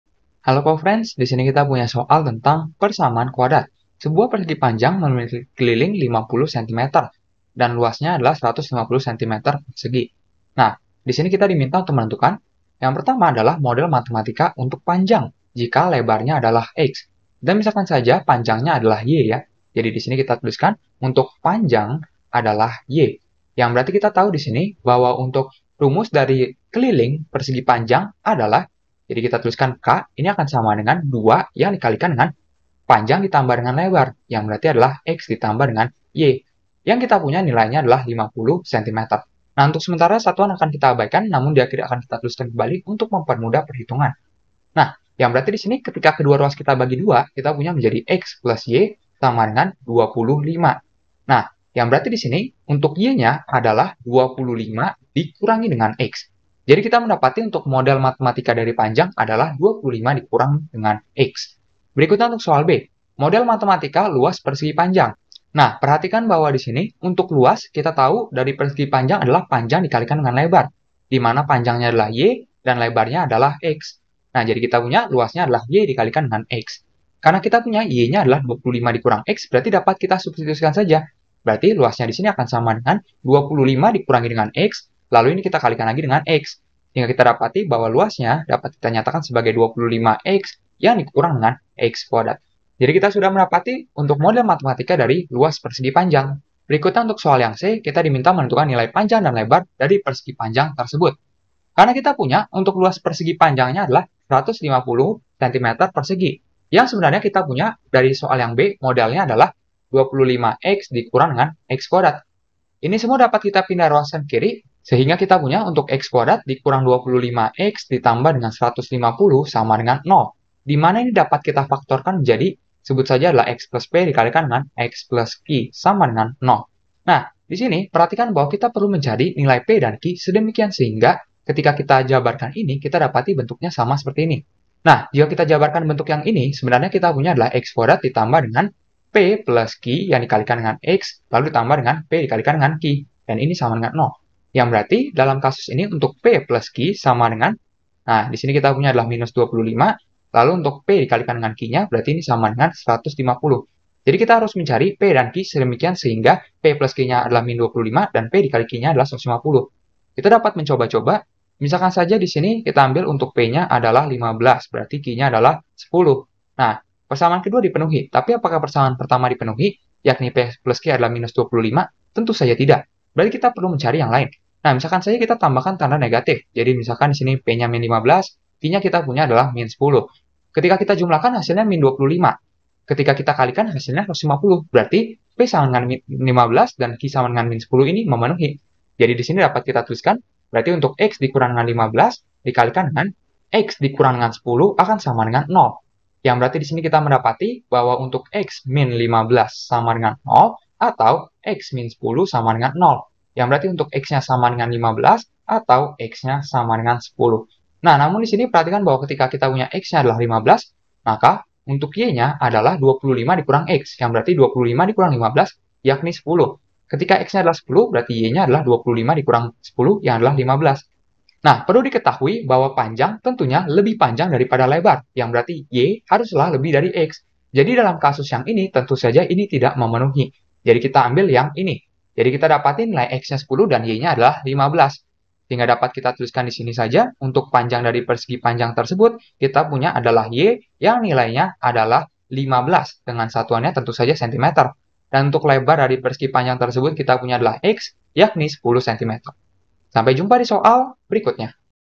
Sebuah persegi panjang mempunyai keliling 50 cm, jika luas persegi panjang 150 cm?. Tentukan Mode...
Sebuah persegi panjang mempunyai keliling 50 cm, jika luas persegi panjang 150 cm?. Tentukan Model matematika panjang, jika lebar = X b Model matematika luas persegi panjang Nilai  panjang dan lebar   persegi   panjang tersebut

Topik atau Materi: Penerapan Sistem Persamaan Linear Dua Variabel - SISTEM PERSAMAAN LINEAR DUA VARIABEL (SPLDV) - ALJABAR - Matematika Kelas 8 SMP -

Sumber Soal: Student Input App (Negative Set) Matematika Kelas 8, 2020 , N/A

00:00 : Bedah Soal
01:00 : Konsep, rumus dan pengertian Penerapan Sistem Persamaan Linear Dua Variabel
01:18 : Langkah penyelesaian soal
03:11 : Jawaban akhir
03:41 : Penutup